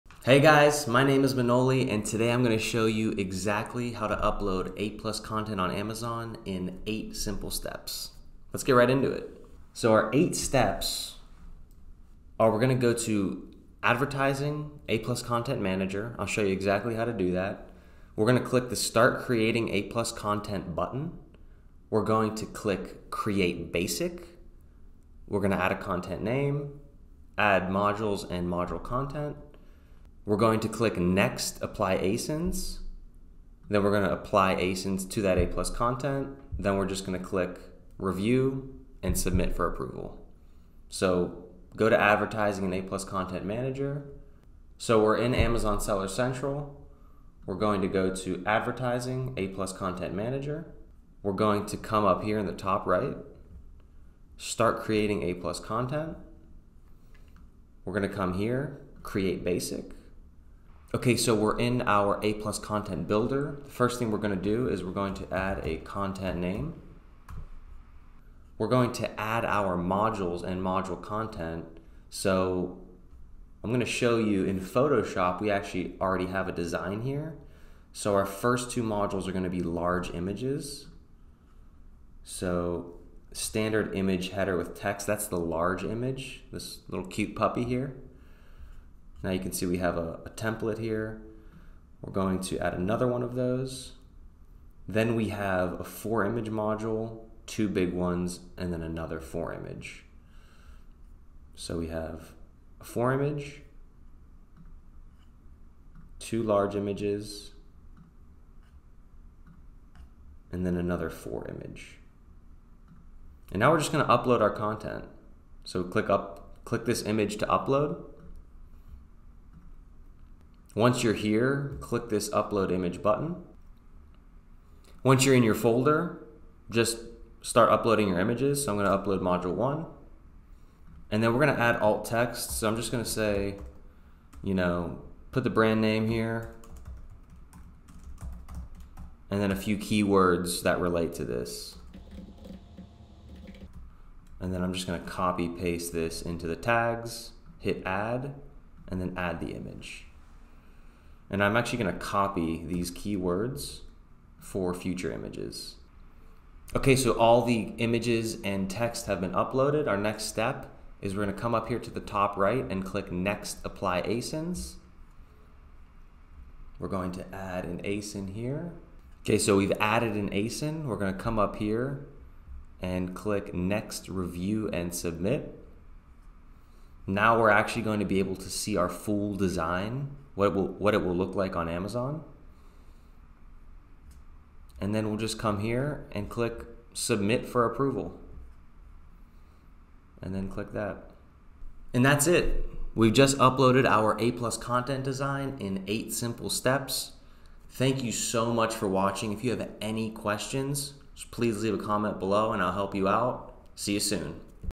How to upload A+ CONTENT on Amazon
How to update your Amazon A+ Content (in 8 simple steps):

1. Go to Advertising → A+ Content Manager
2. Click “Start Creating A+ Content”
3. Click “Create Basic”
4. Add a content name
5. Add modules and module content
6. After creating your design, click “Next: Apply ASINs”
7. Apply ASINs
8. Click “Next: Review & submit” and “Submit for approval”